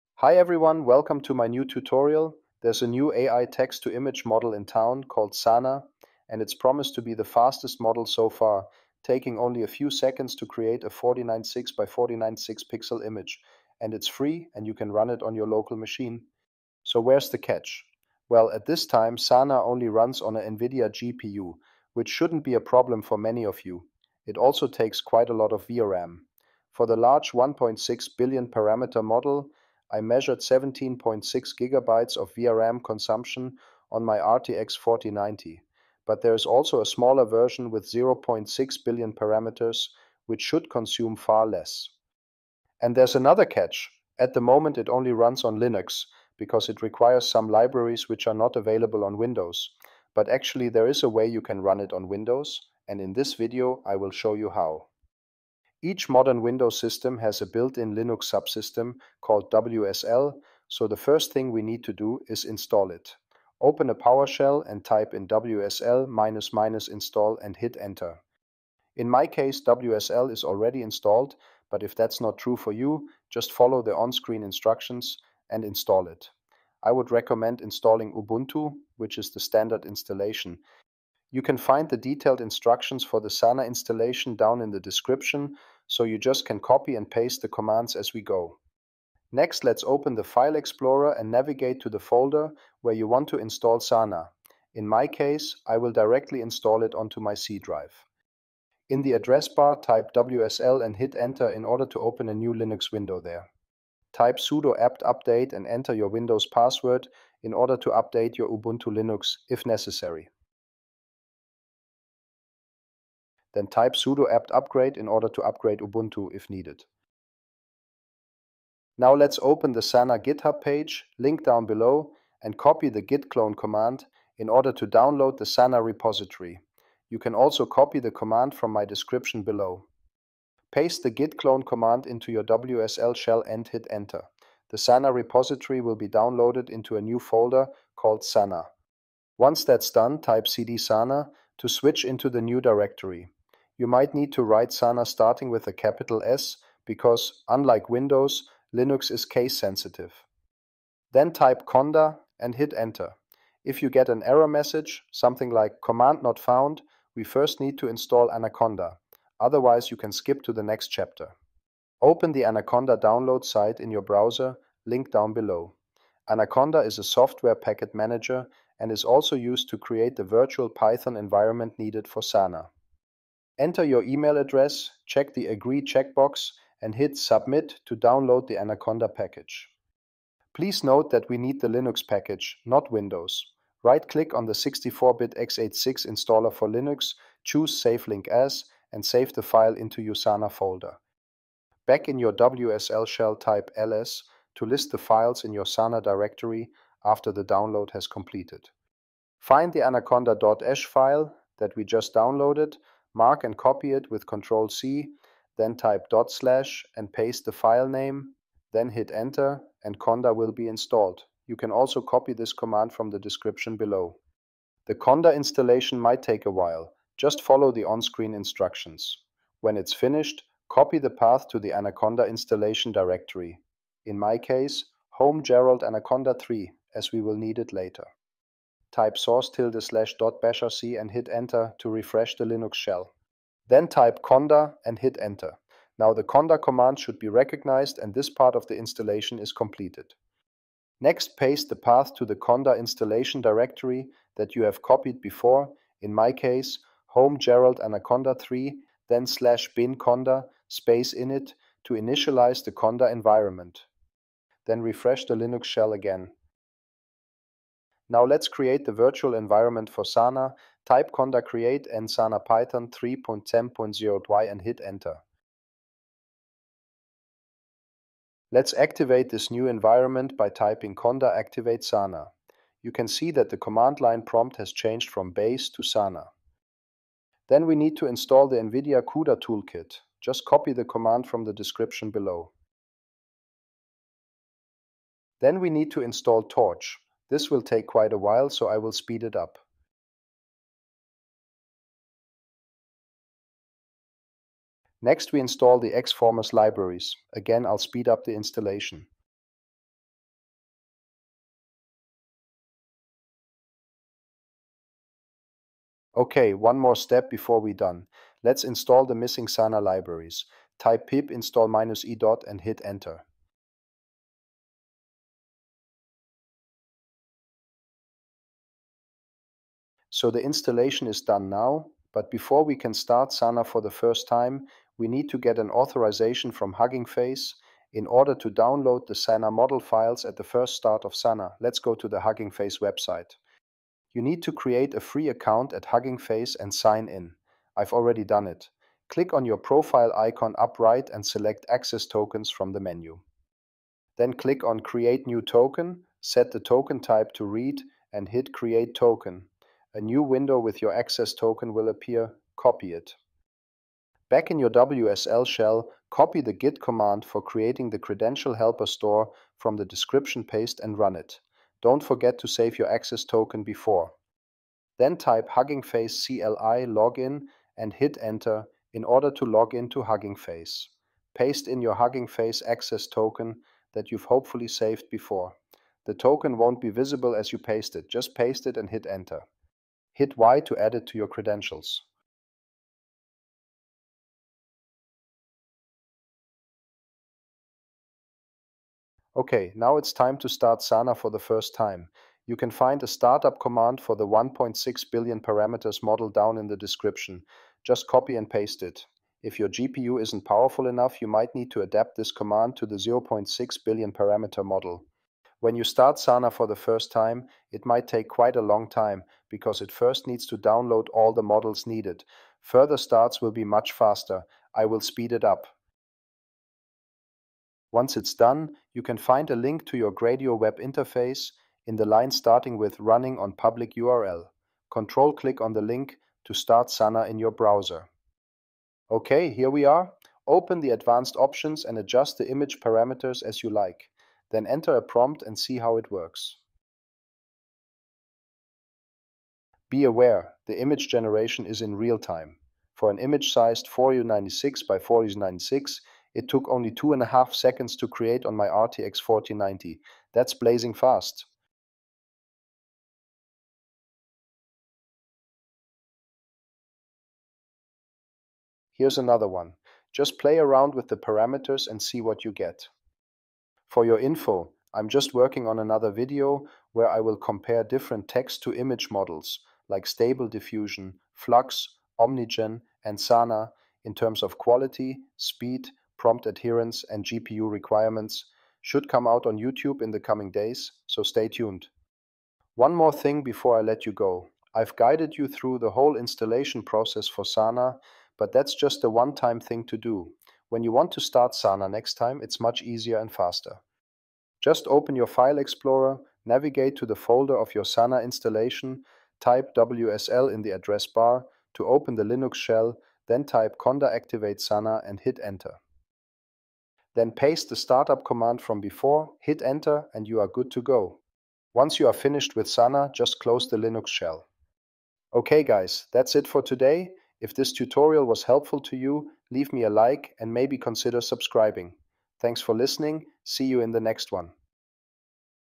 Running SANA Text-to-Image locally on Windows
SANA is the new, blazing-fast Text-to-Image model from NVIDIA, where you can create images with a size up to 4096x4096 pixel in just a few seconds. While an Online-Demo has been out for a little while, it's now possible to run it on a local machine with an NVIDIA-GPU.

By this time, it's only meant to be for Linux, but in this tutorial I will show you how to run it on Windows, using the built-in Windows Subsystem for Linux (WSL). The installation process will be explained step by step and you can find the commands for each step further down below.

Hope it can be helpful to you!

00:00 Intro, Requirements
00:55 Installing the Windows Subsystem for Linux (WSL)
01:29 Cloning SANA from Github
02:41 Installing Conda on WSL
04:16 Create and activate the virtual environment for SANA
04:36 Installing Cuda, Torch, xFormers and SANA libraries
05:20 Create and install Huggingface-Token
06:30 Starting SANA for the first time
07:10 Creating images with SANA
08:03 How to start SANA after the installation

Installation Process:
1.) Installing the Windows Subsystem for Linux

in Windows Powershell type:
wsl --install

(choose Ubuntu Standard installation)

2.) Open WSL in the directory, where you want to install SANA
in the WSL-shell update & upgrade WSL:

cd Sana

into your SANA directory and run it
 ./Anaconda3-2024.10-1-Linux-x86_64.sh

copy installation path (in my case: home/gerald/anaconda3)
reload wsl-shell:
source ~/.bashrc

initialize conda (use your own installation path!):
/home/gerald/anaconda3/bin/conda init

reload wsl-shell:
source ~/.bashrc

5.) Create and activate virtual environment for SANA:
conda create -n Sana python=3.10.0 -y
conda activate Sana

6.) Install required libraries:
conda install -c nvidia cuda-toolkit -y
pip install torch
pip install -e .

git config --global credential.helper store
huggingface-cli login
(paste token)

8.) Start SANA (1.6GB model):
DEMO_PORT=15432 \
python app/app_sana.py \
      --config=configs/sana_config/1024ms/Sana_1600M_img1024.yaml \
      --model_path=hf://Efficient-Large-Model/Sana_1600M_1024px/checkpoints/Sana_1600M_1024px.pth

9.) Restart SANA subsequently:
open wsl in SANA directory and type

10.) Have fun!
conda activate Sana
DEMO_PORT=15432 \
python app/app_sana.py \
      --config=configs/sana_config/1024ms/Sana_1600M_img1024.yaml \
      --model_path=hf://Efficient-Large-Model/Sana_1600M_1024px/checkpoints/Sana_1600M_1024px.pth